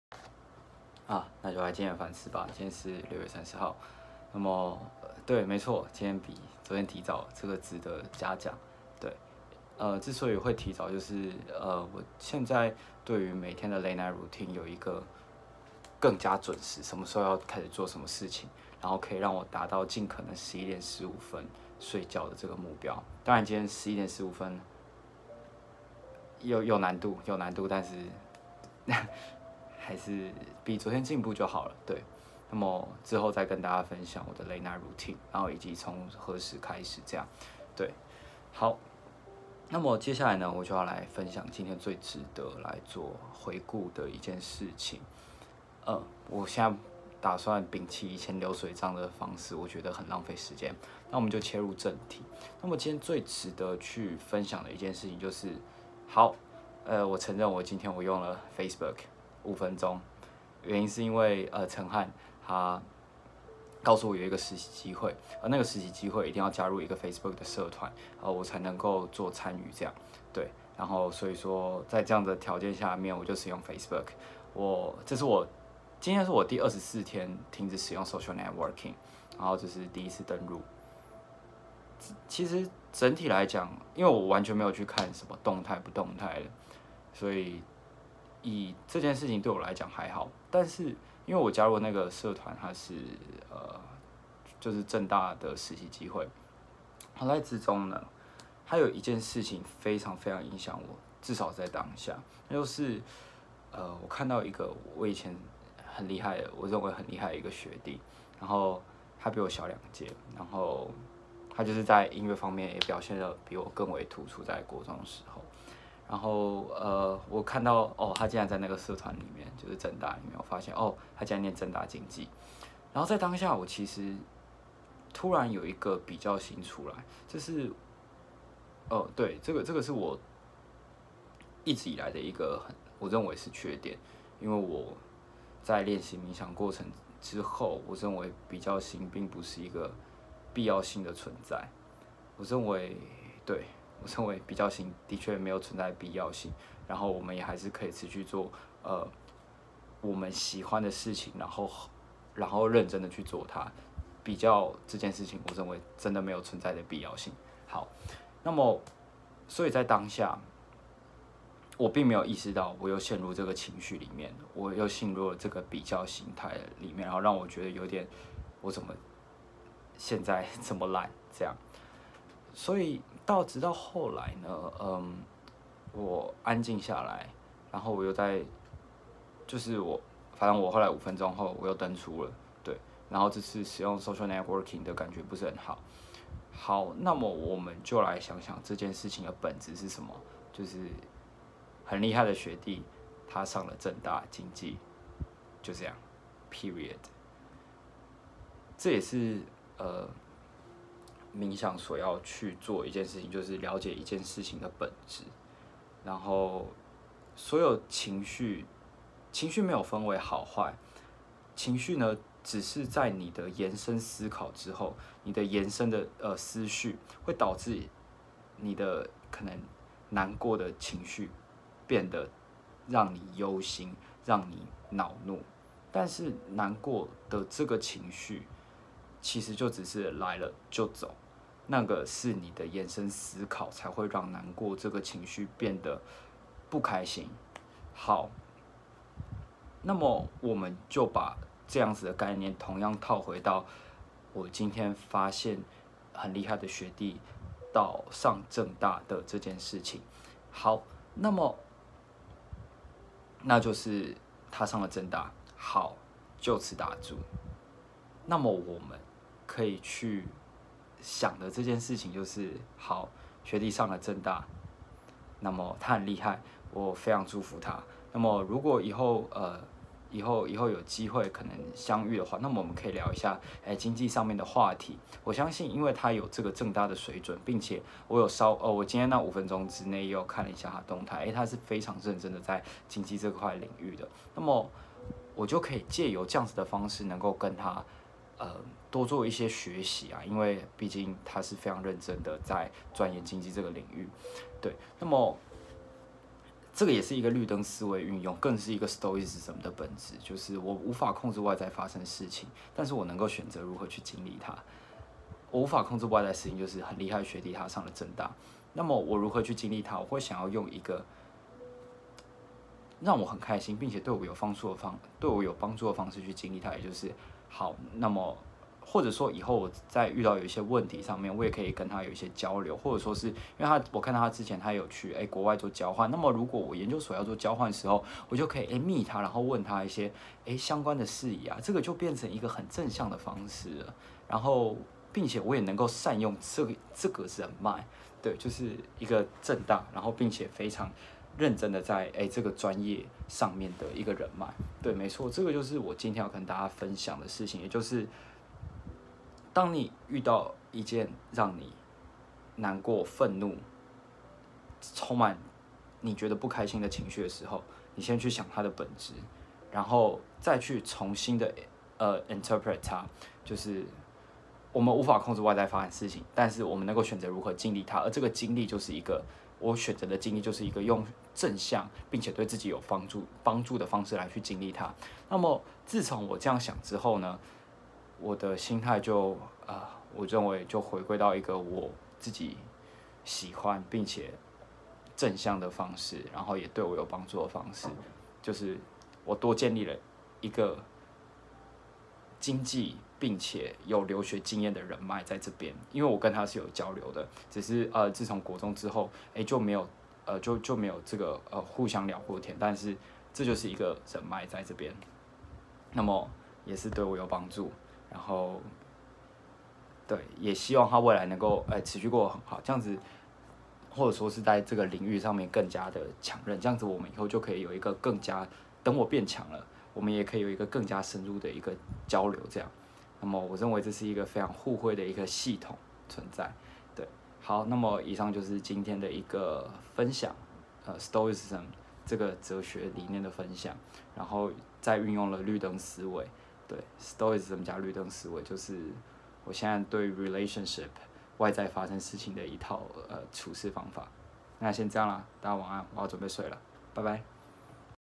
每日反思 - 嫉妒比較心的應對心態，運用 Stoicism+ 綠燈思維 #20
今天分享的事情是嫉妒比較心態，是我長久無法釋懷的部分，我個人沒有很喜歡，因為我認為這種心態會使我自己過於在意某個領域當下的表現，而非以整個人生的大框架和未來長期的可塑性進行思考。

之後我運用了 stoicism 的概念以及綠燈思維，做了一個正向的延伸思考，這樣想就好多了，並且日後也對我會有實質的幫助。

最後，希望日後減少流水帳，每日選取一件最重要的事情，跟大家分享。